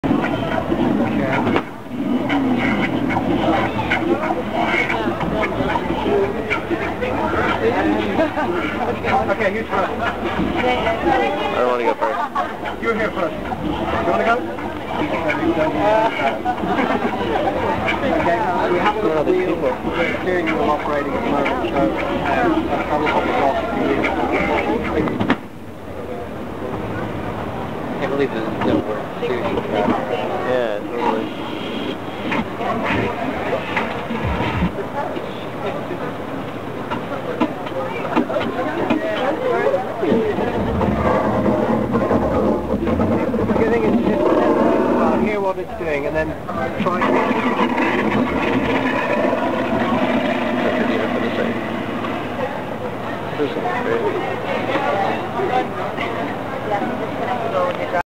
Dublin Art and Technology Association - Event 5 - Datsuncredible
This is a live demo of the Datsuncredible car that is hooked up to digital music software.